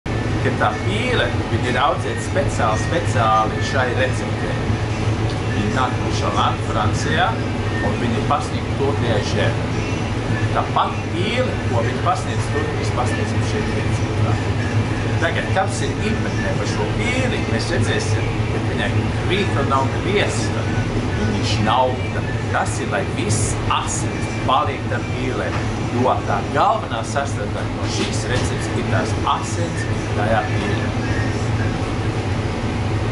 Présentation de la recette du "canard à la presse" par le chef letton Mārtiņš Rītiņš
Présentation (en letton) de la recette du canard à la presse par le chef Mārtiņš Rītiņš (Restaurant Vincents) avant le dîner "Goût de France / Good France" organisé à la résidence de l'ambassade.
Šefpavārs Mārtiņš Rītiņš (restorāns „Vincents”) prezentē (latviski) „canard à la presse” recepti pirms „Goût de France / Good France” vakariņām Francijas vēstniecības rezidencē.